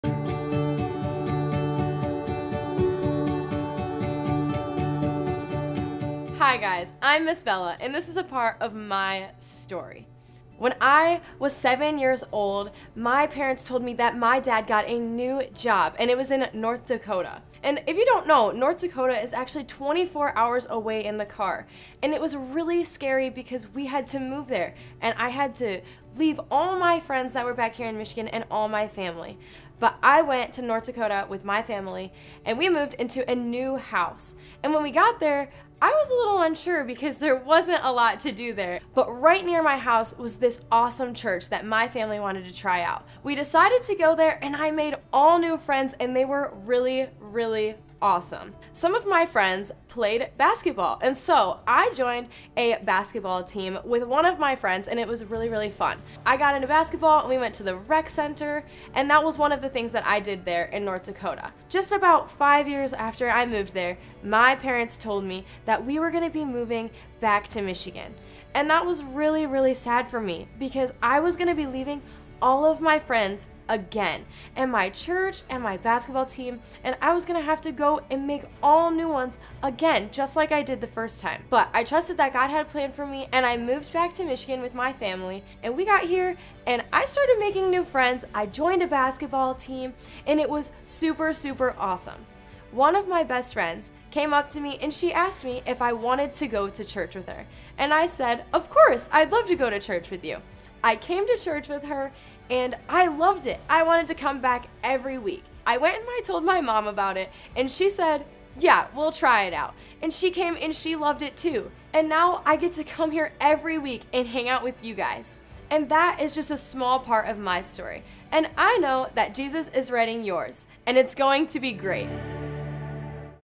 My Story | Here We Go Again
Life is a story filled with purpose, grace and adventure. Listen as our leaders share a small part of their story & how God has impacted their journey. Jesus is writing your story & it’s going to be GREAT.